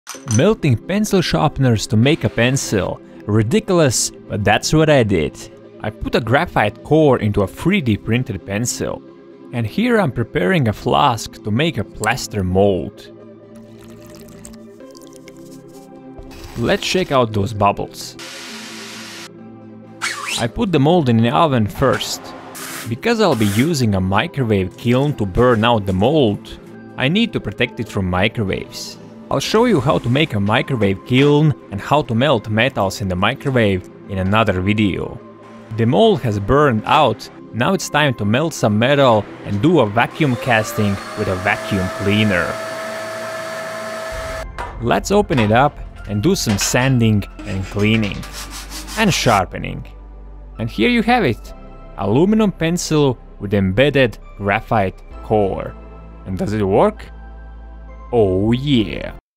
Aluminium Pencil
This is a video about melting pencil sharpeners to make an aluminium pencil with embedded graphite core.